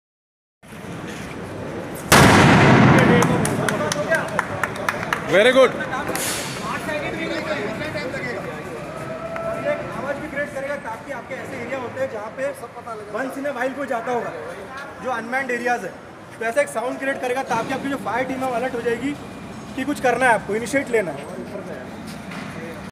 APFE Demo at Triveni Engineering and Industries Limited | Sugar Group | Part-3 | Meerut | U.P.
APFE demonstration was conducted. These automatic fire extinguishers expel on their own in case a fire starts. It doesn't harm the things around it. Also the sound you hear when its explodes its just because to let people be aware that fire.
1.Automatic ABC Ceiling Mounted Modular
2. Automatic Portable Fire Extinguisher (APFE)

*Automatic Activation
*100 % recyclable
*Light in Weight
*0 % False alarm
*Compact in size
*Not harmful to humans, animals & environment
*Non-Toxic
*No Maintenance required
*Non-hazardous
*Any age group can activate
*Biodegradable
*Ozone Friendly
*5 Years Warranty